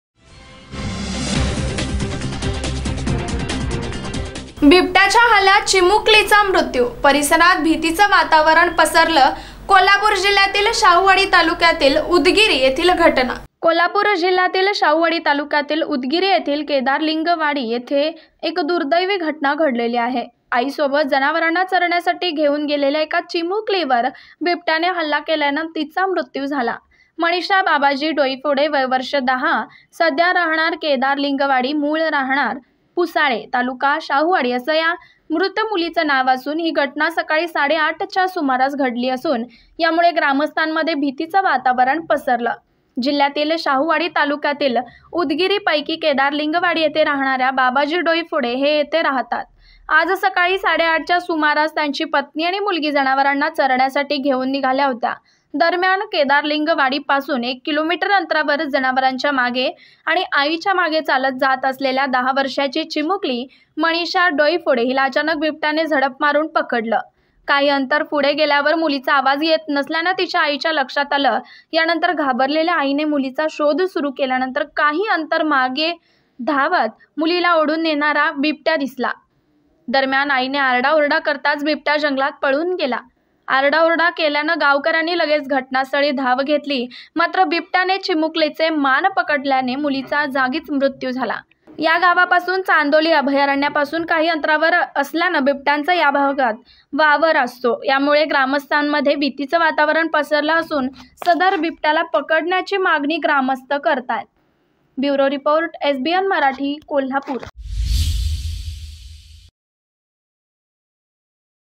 KOLHAPUR | बिबट्याच्या हल्ल्यात चिमुकलीचा मृत्यू; परिसरात भीतीचे वातावरण
बिबट्याच्या हल्ल्यात चिमुकलीचा मृत्यू; परिसरात भीतीचे वातावरण, कोल्हापूर जिल्ह्यातील शाहूवाडी तालुक्यातील उदगिरी येथील घटना